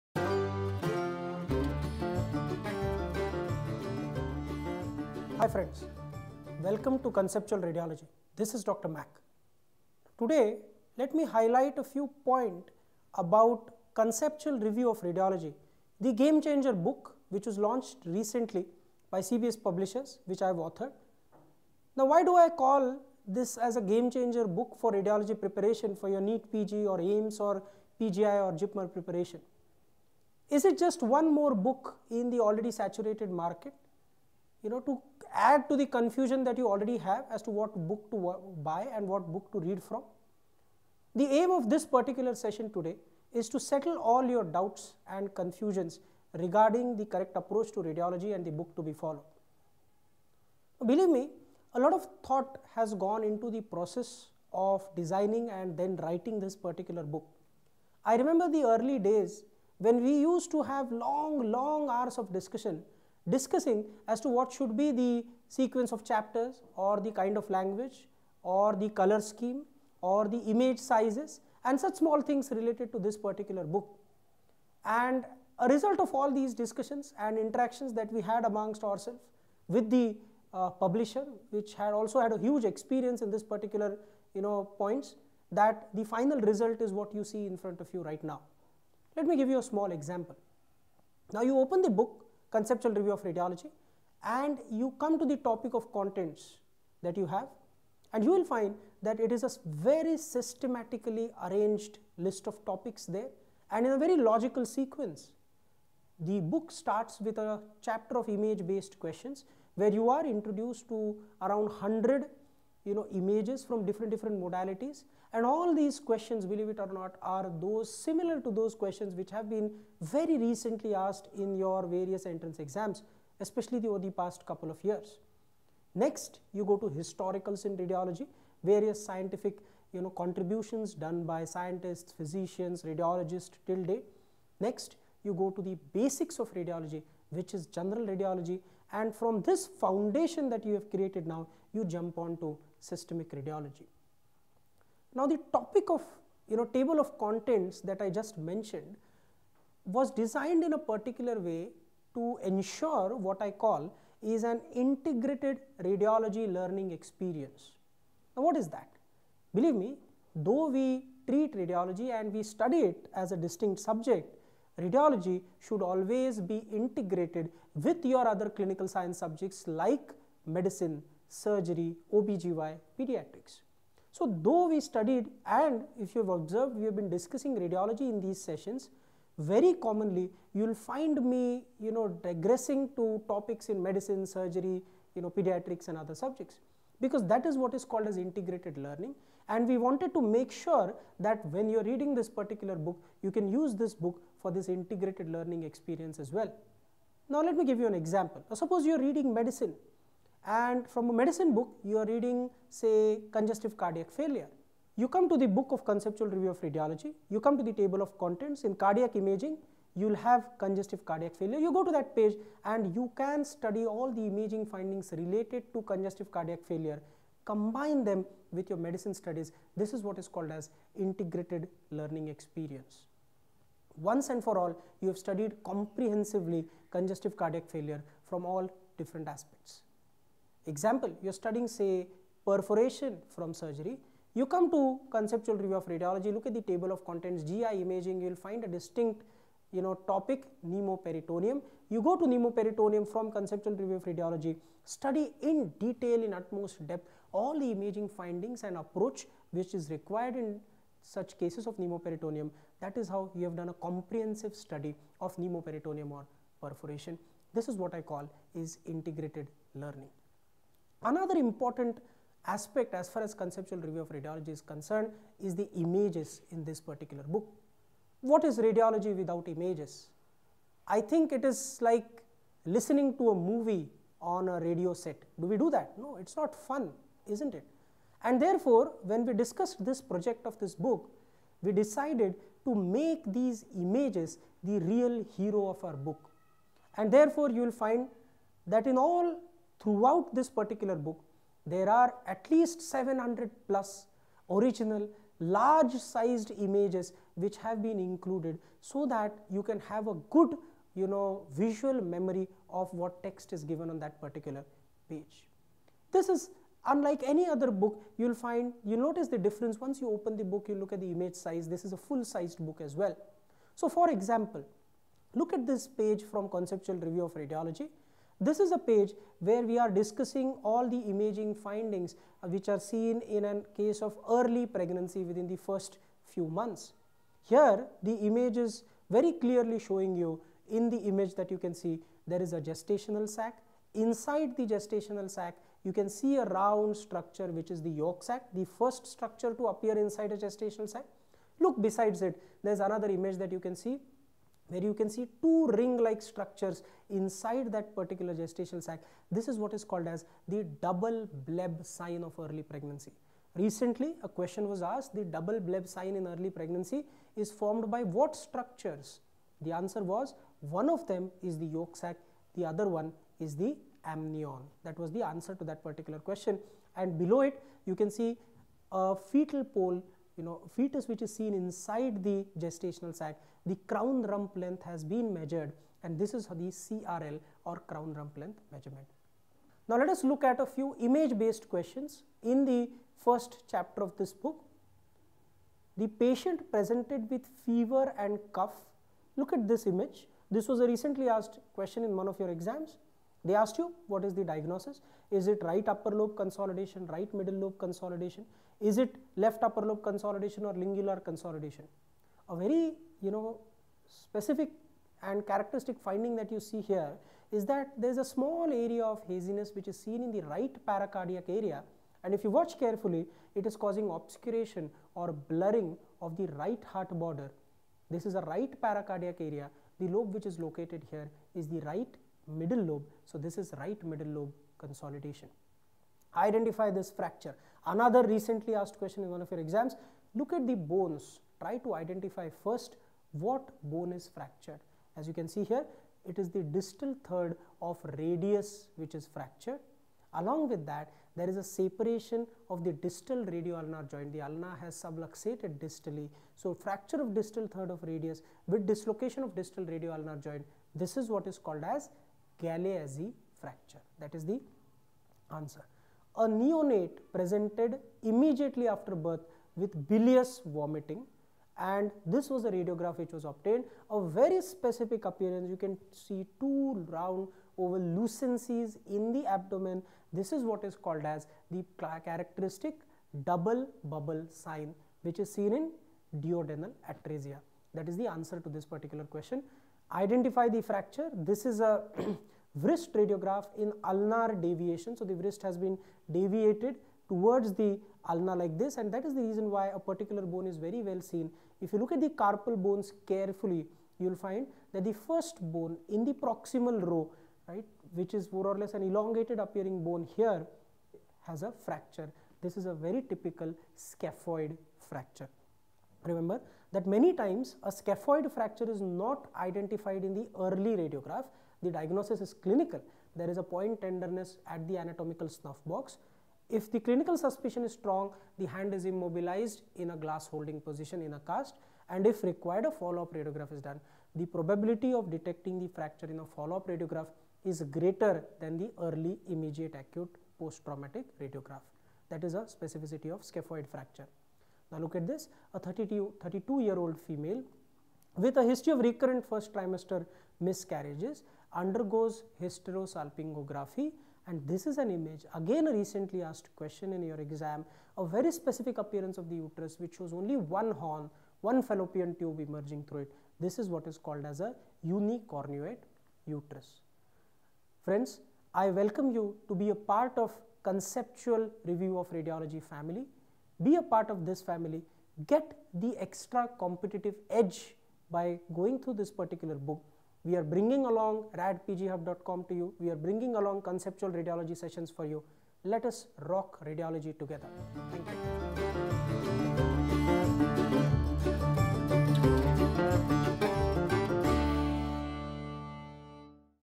CONCEPTUAL REVIEW OF RADIOLOGY - GAME-CHANGER BOOK FOR PGMEE
CRR - 1ST FULL SIZED, MULTI-COLOR, IMAGE ORIENTED RADIOLOGY BOOK WITH A UNIQUE CONCEPTUAL APPROACH HIGHLIGHTED THROUGH 100+ CONCEPT BOXES. THIS VIDEO HAS IMAGE BASED QUESTION DISCUSSION FROM CHAPTER 1.